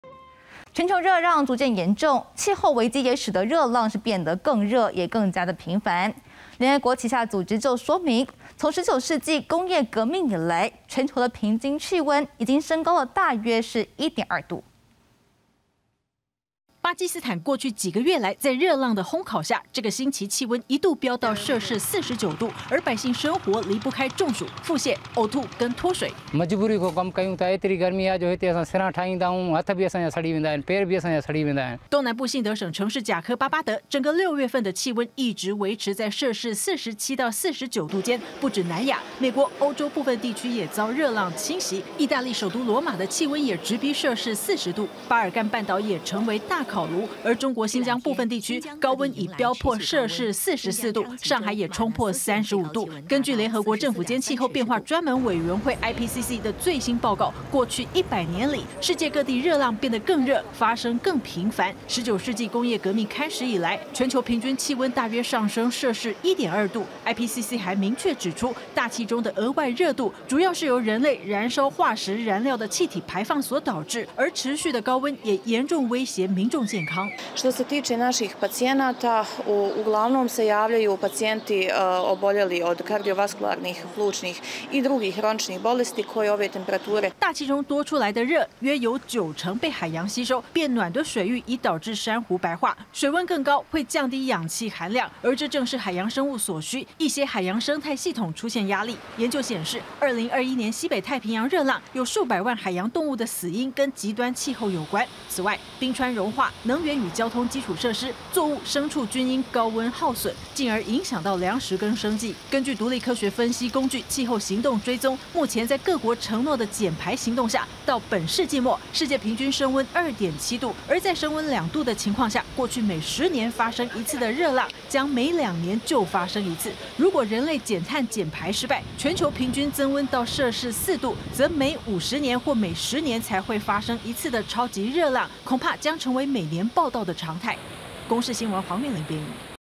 IPCC：全球百年增攝氏1.2度 未來熱浪頻率恐上升｜20220630 公視晚間新聞
全球熱浪越來越嚴重，氣候危機使得熱浪更熱、更頻繁，聯合國旗下的「政府間氣候變化專門委員會」（IPCC）指出，自19世紀工業革命以來，全球平均氣溫升高約攝氏1.2度，而禍首是人類燃燒化石燃料的氣體排放。